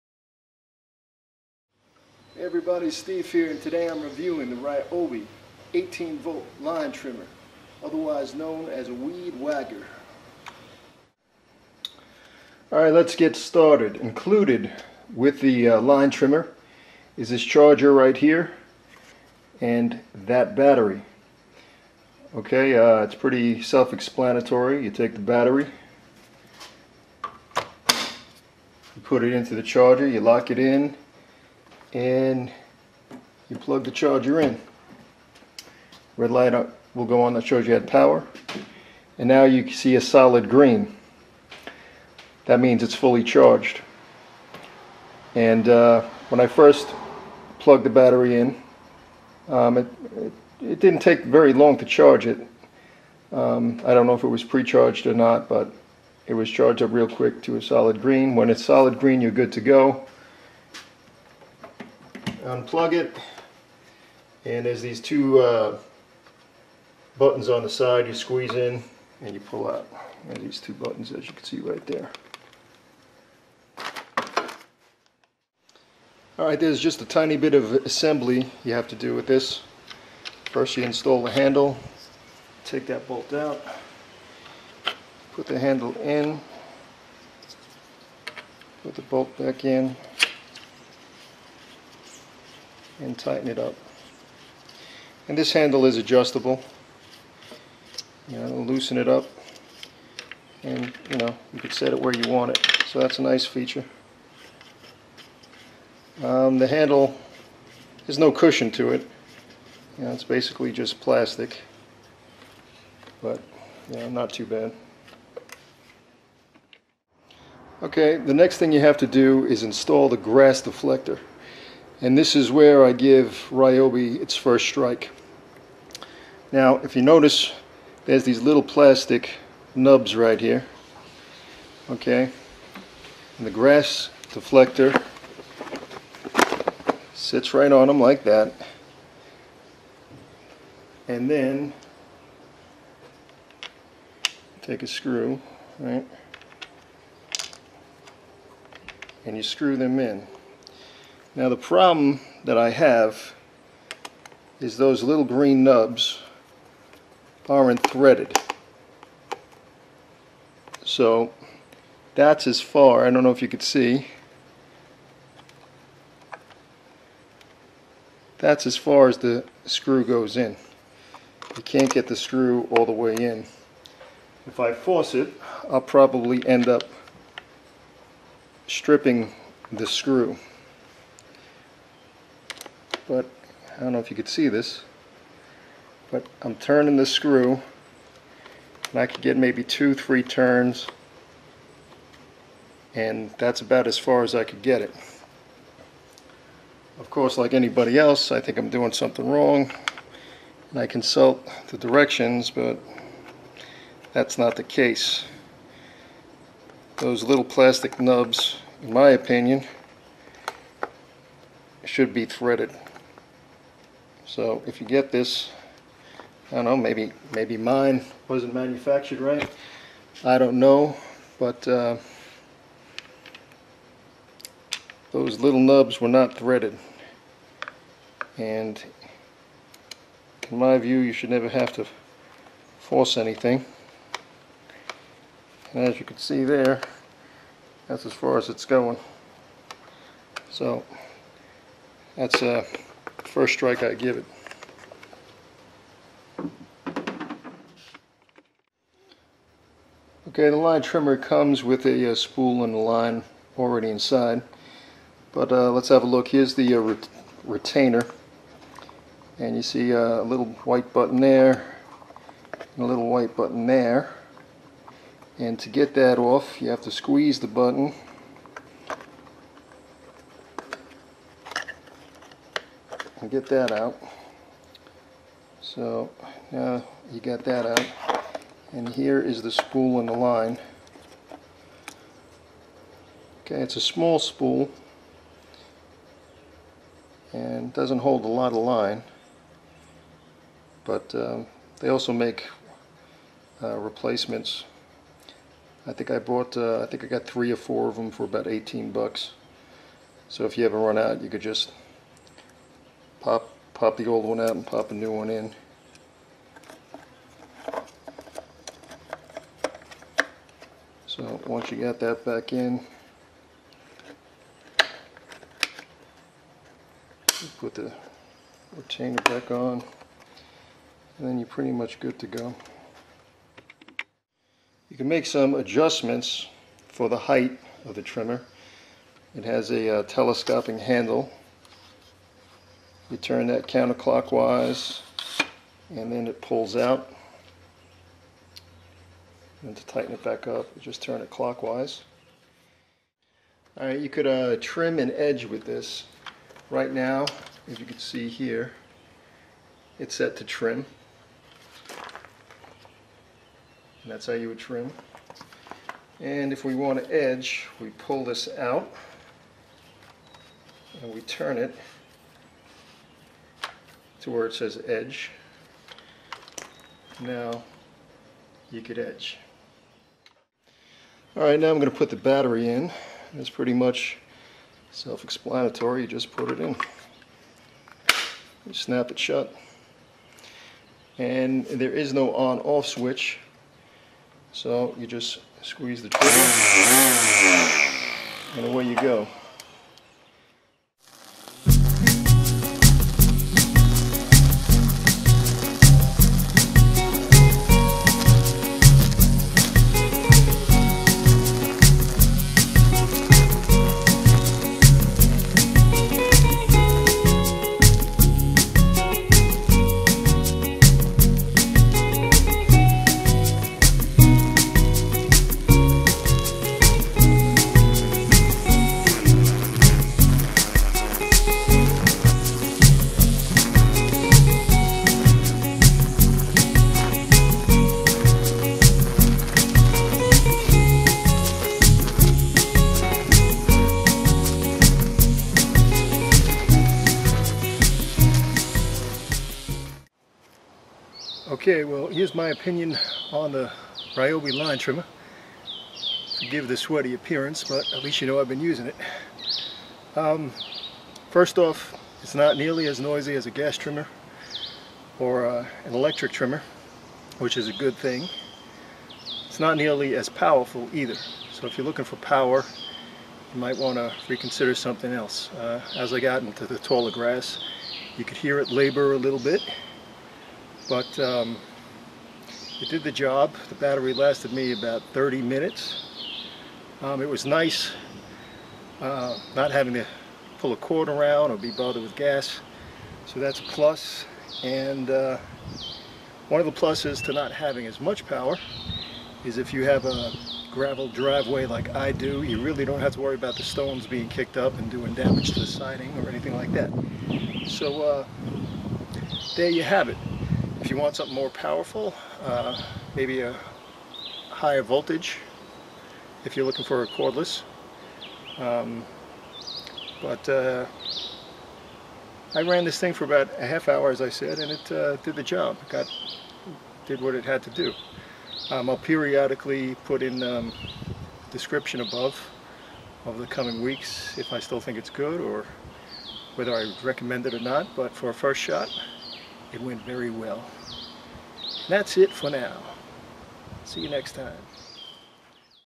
RYOBI 18 VOLT LINE TRIMMER REVIEW/DEMO
Update 5/12/24- Still working. So, that's 10 years. Not bad for $89.

Update 3/20/22- Still works awesome.

Update July 7, 2021- Still works great after 7 years.

April 20, 2019- Still working great after 5 years. Still using original battery. More updates
below.

4/8/18- Still works great. Used it for an hour straight.

8/11/17- Still works great. Just had to buy a new spool cap for it, $5 at Home Depot.

5/1/17- Another year has gone by and it still works great.

5/19/16- Update... Ryobi trimmer still working great.

June 18, 2014- My review and out of the box demo of the RYOBI 18 volt line trimmer. Purchased from Home Depot for $89.99. Battery and charger included.